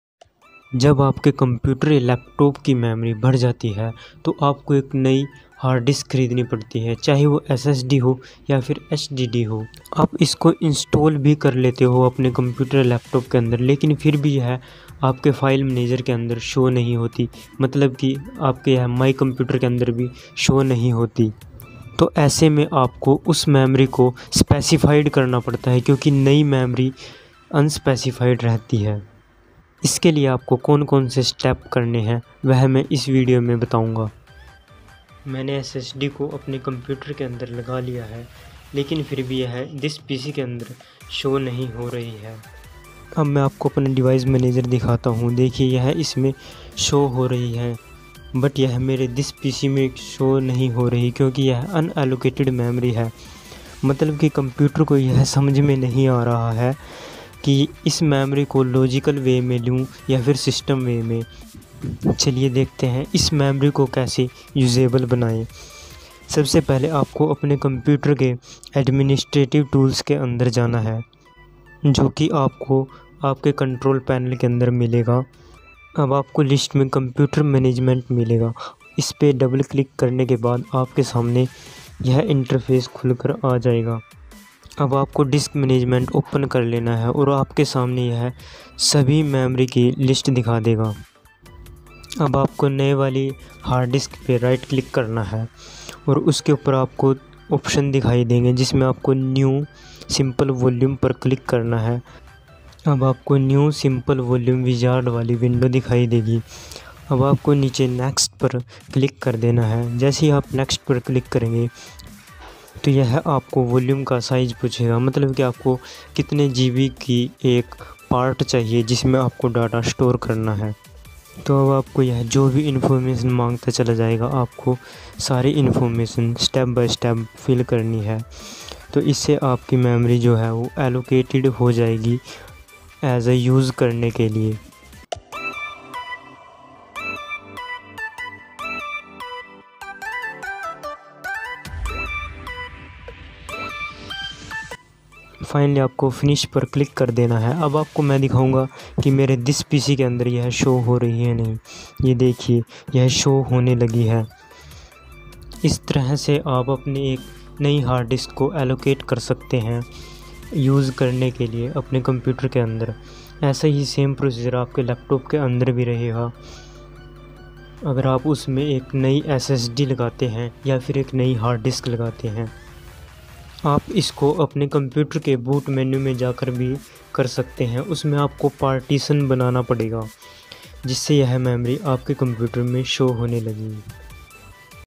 my pc not show new SSD or hard disk || new ssd settings || new computer settings
is video me mai aapko btauga ki aap apni new disk drive ko kese allocated kar sakte hai
step by step full  process
SSD ke fayde
SSD Kese lgate hai